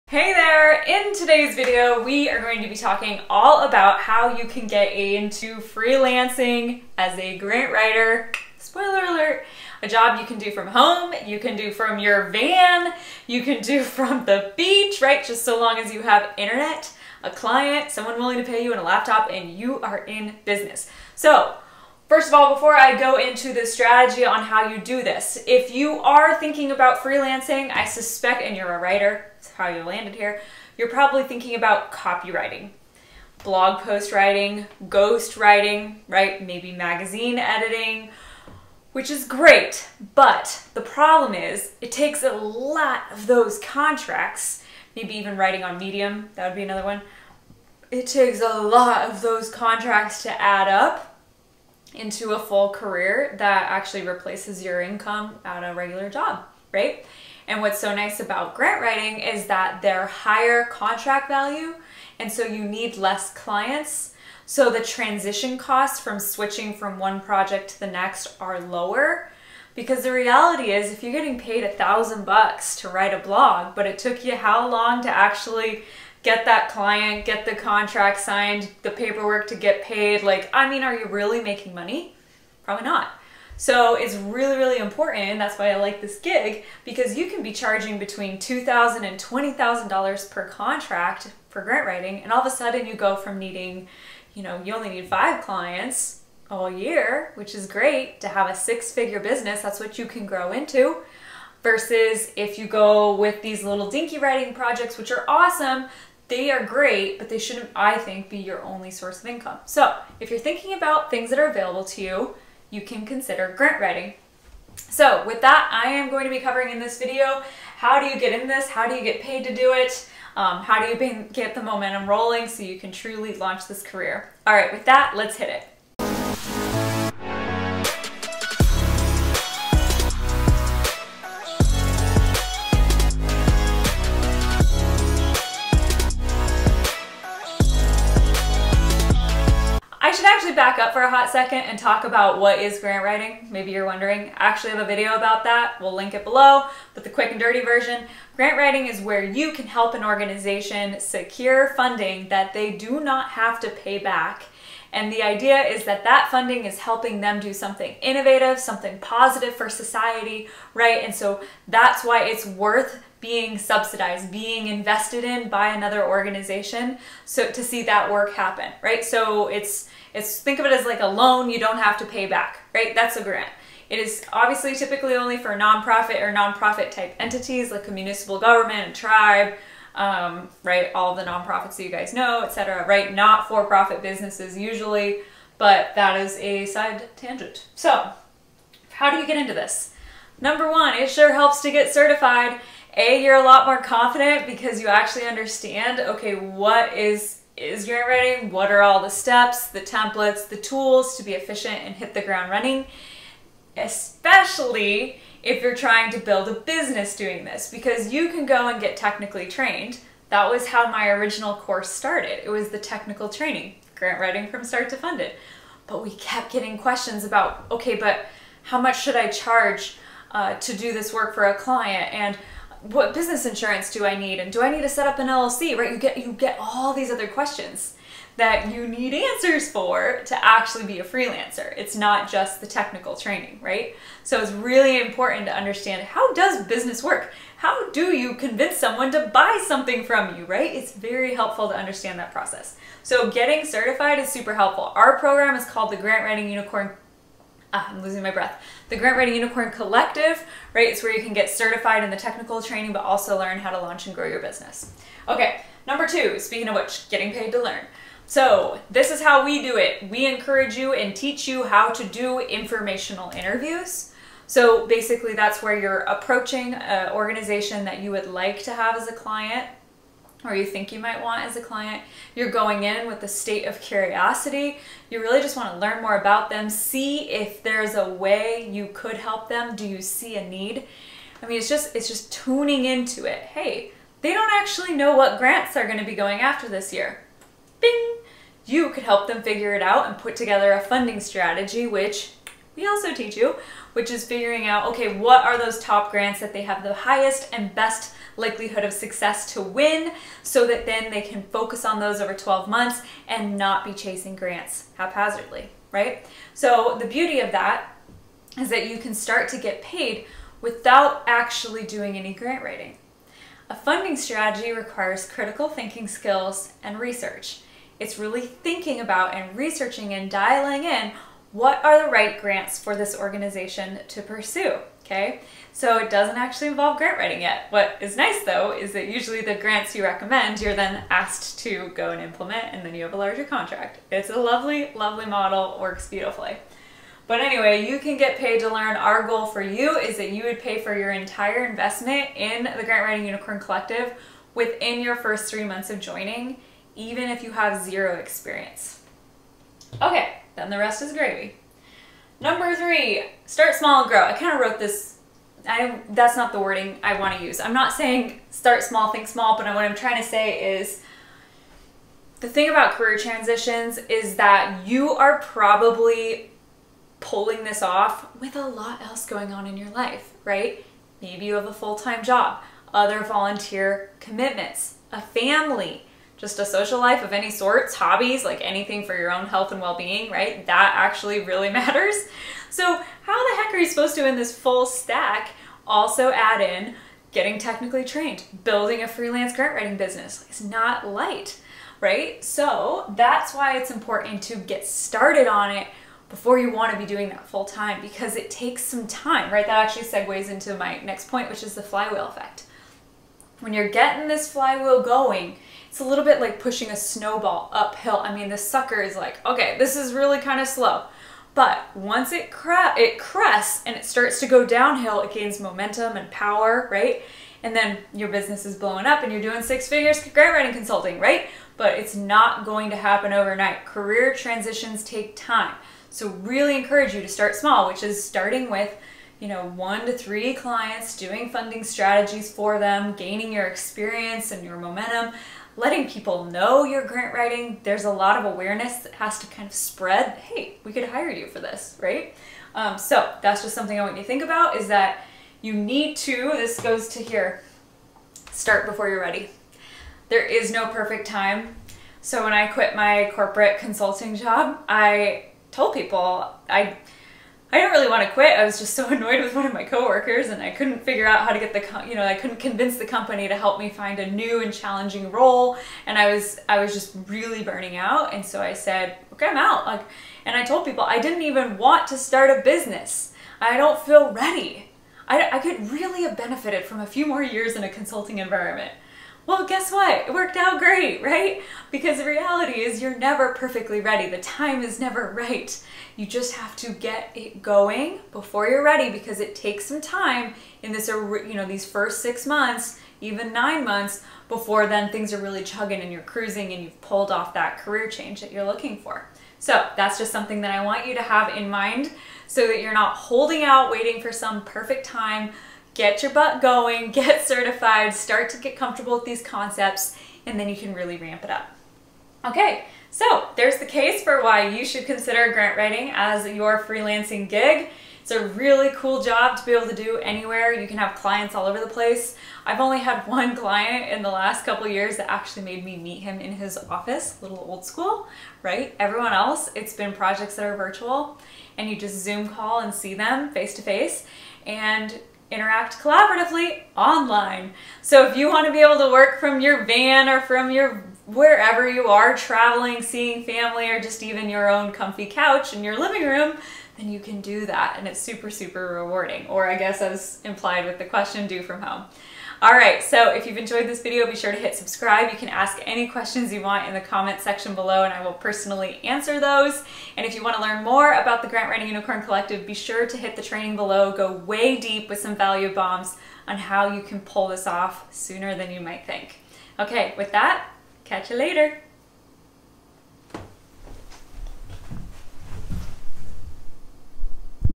Freelance Writing Jobs for Beginners and Jobs You Can Do From Home (Grant Writing!)
Design a career that supports your lifestyle goals. We will show you how you can get the                    ⚡️ momentum running⚡️ to break into grant writing. We recommend the first most important step be: getting your Grant Writing Certificate! Tune in for more pointers.

Free training on how to build a career in grant writing

Getting paid to learn grant writing is possible. We recommend getting started slowly while embracing a growth mindset as your flywheel gains momentum.

🧡  The Team at Learn Grant Writing

TIMESTAMPS:
00:00 Freelance Writing Jobs For Beginners And Jobs You Can Do From Home
02:20 What Is Grant Writing?
03:08 How Do You Get Into Grant Writing?
04:16 Getting Paid To Learn
05:53 Start Small With A Growth Mindset
06:42 Flywheel Effect
07:27 Start Before You Are Ready